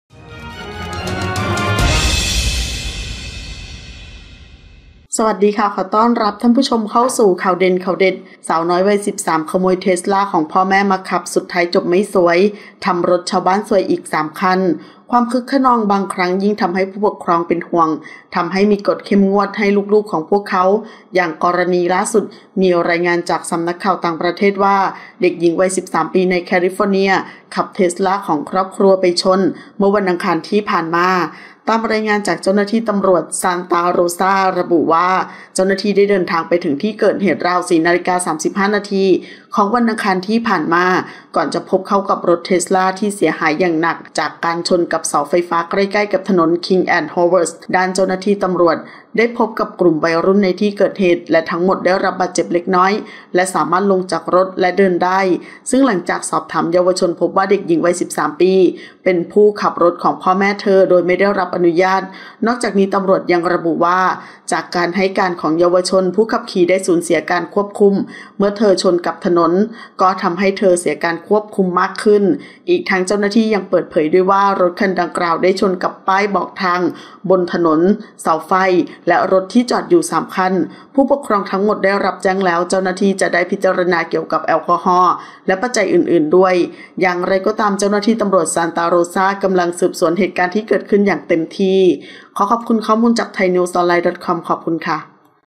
สาวน้อยวัย 13 ขโมยเทสลาพ่อแม่มาขับ สุดท้ายจบไม่สวย ทำรถชาวบ้านซวยอีก 3 คัน
00:00 - Intro
00:10 - News
01:00 - Minute